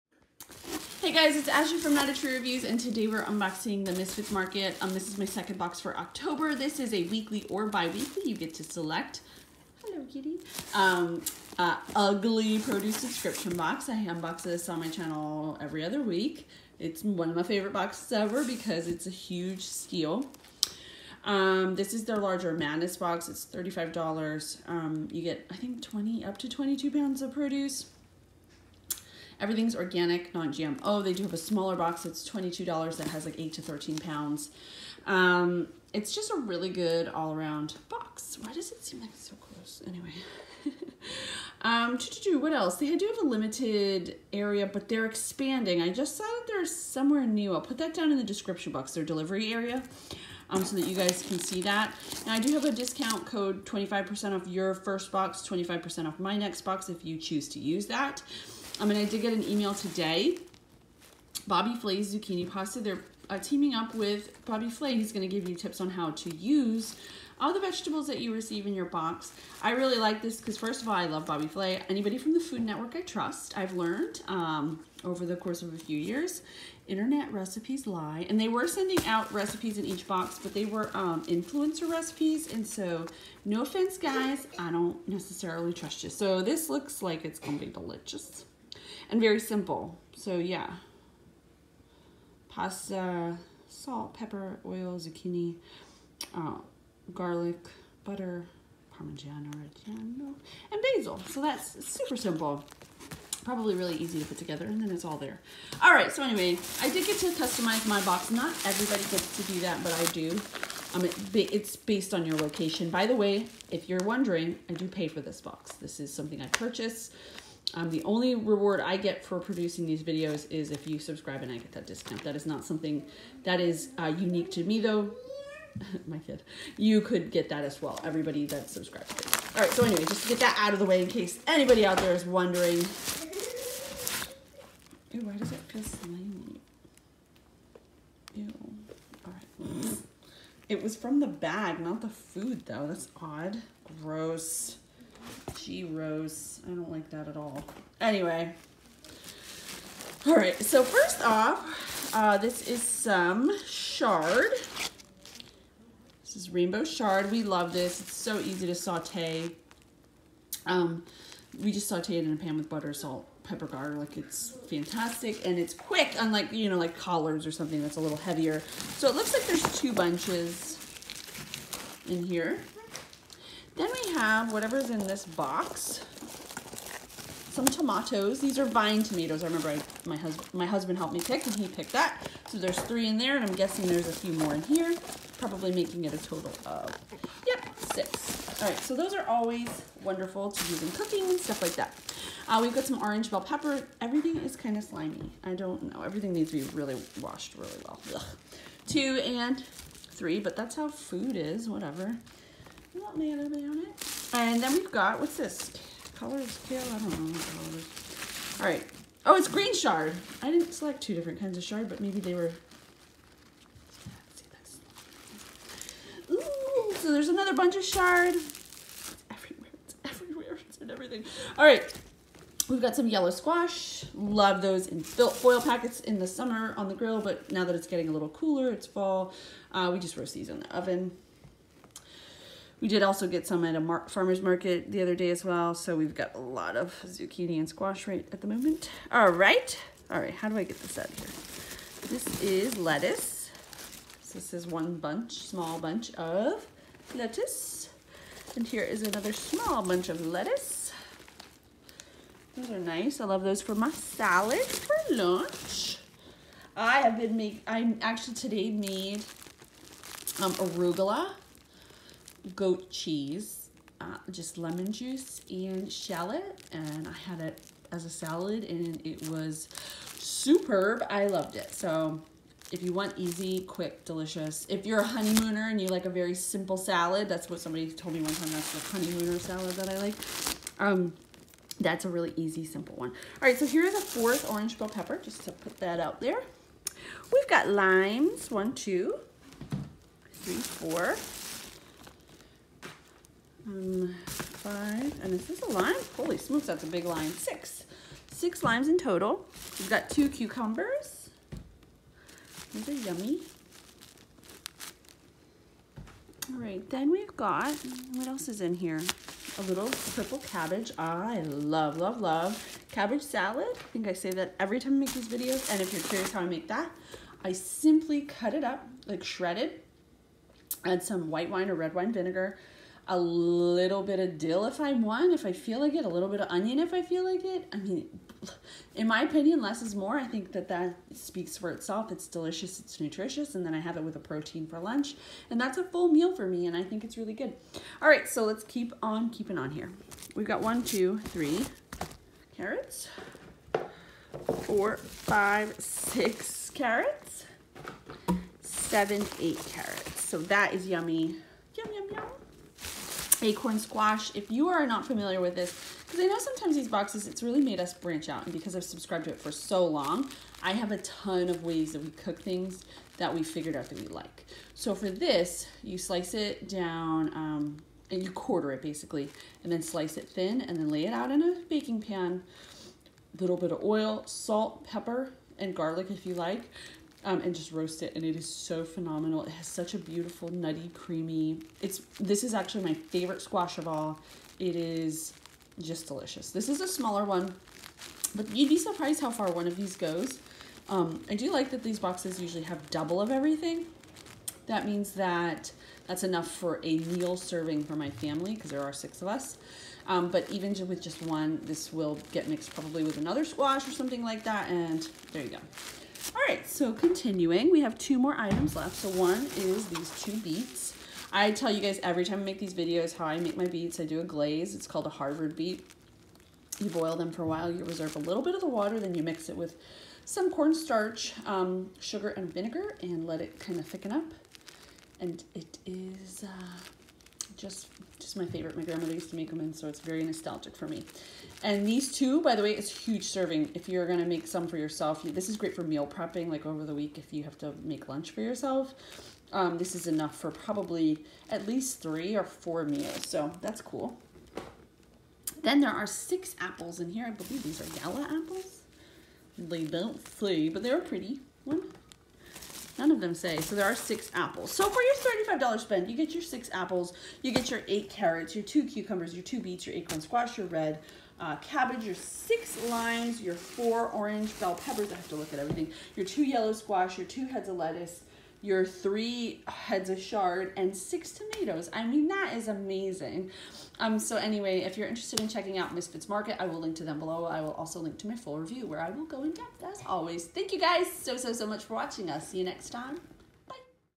Misfits Market Unboxing + 25% OFF
Misfits Market is a weekly or bi-weekly ugly produce subscription box. You’ll have the ability to select from either the Mischief Box which contains 8-13 pounds of produce for $22 or the Madness (what I’m reviewing) that contains 18-23 pounds of produce for $35. Select areas are able to customize their boxes.